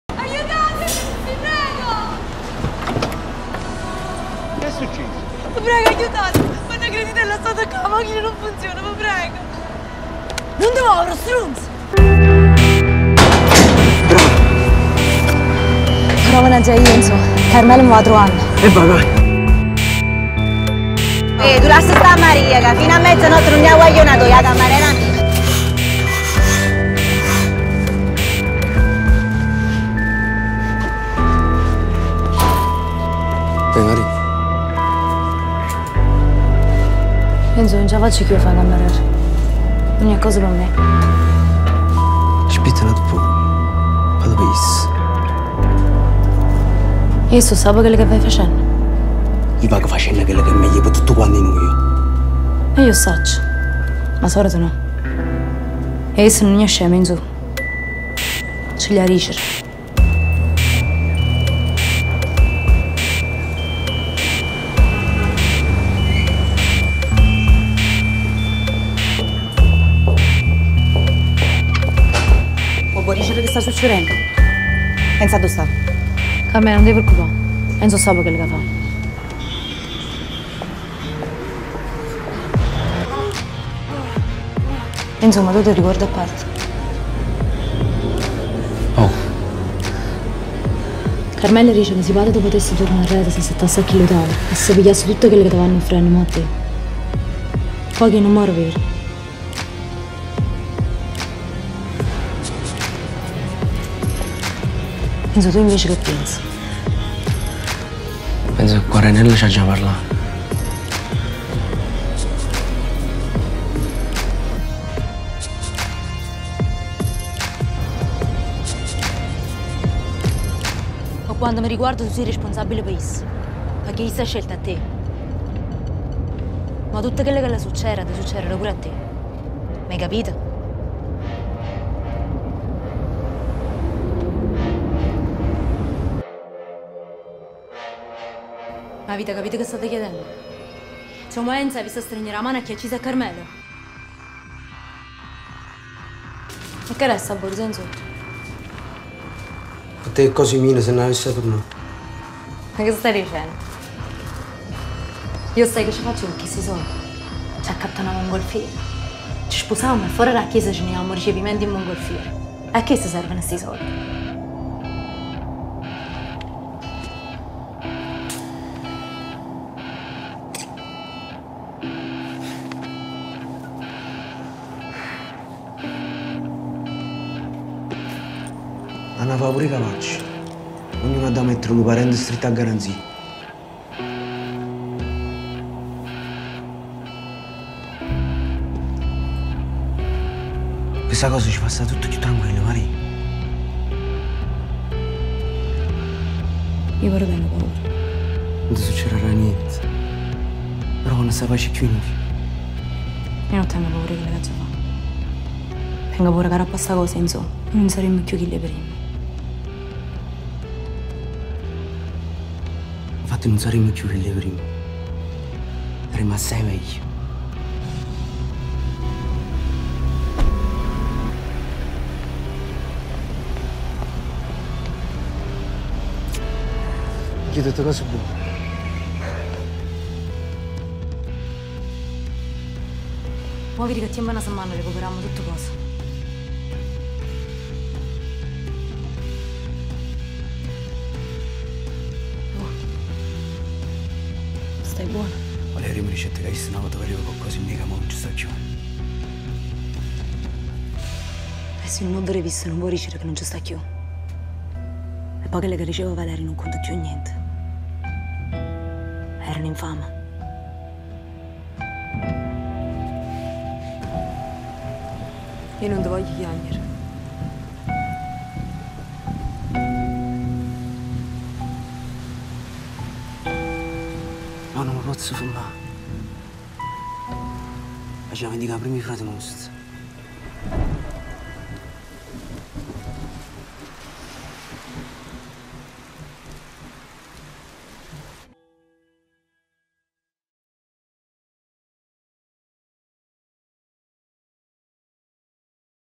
Omaggio a Gina Amarante - Social World Film Festival 2019 [focus Gomorra]
Omaggio all'attrice Gina Amarante (Maria in Gomorra la serie), ospite d'eccezione del Social World Film Festival 2019.

Editor: Salvatore Maglione